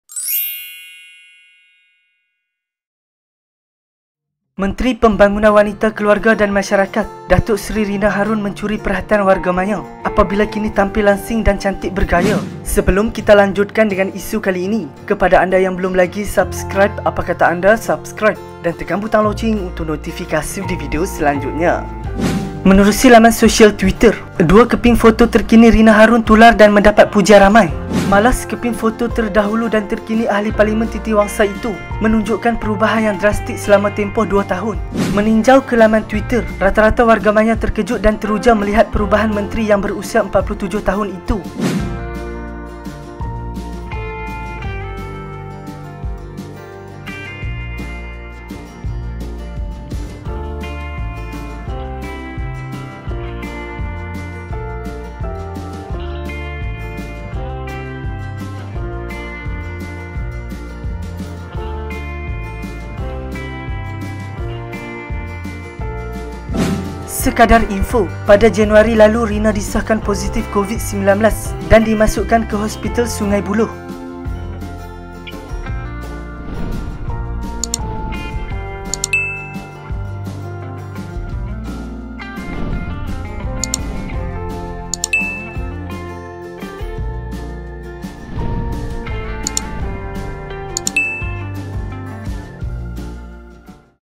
TERKEJUT NETIZEN! Rina Harun Buat Kemunculan Dengan Perubahan Drastik?
Menteri Pembangunan Wanita, Keluarga dan Masyarakat, Datuk Seri Rina Mohd Harun menjadi perhatian wargamaya apabila kini tampil langsing dan cantik bergaya.